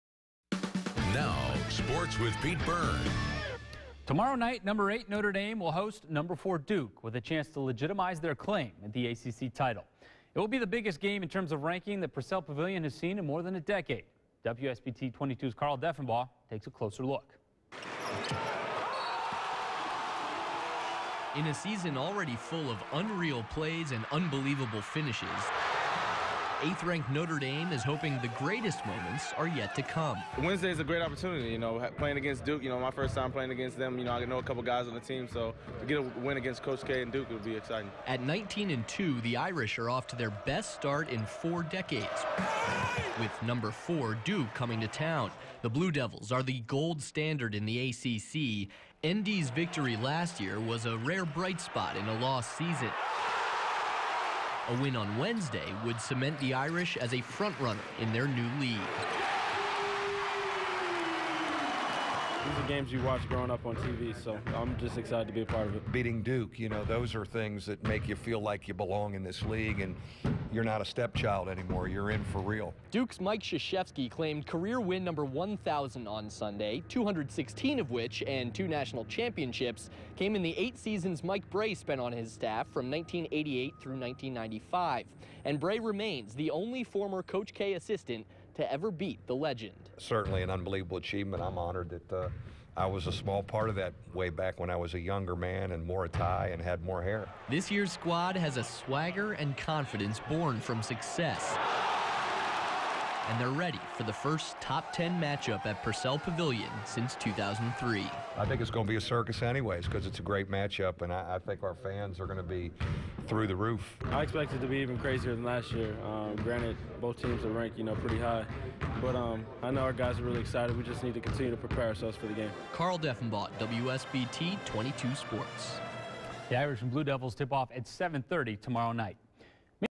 Irish prepare for Top 10 match-up
In a season full of unreal plays and unbelievable finishes, eighth-ranked Notre Dame is hoping the greatest moments are yet to come. _________________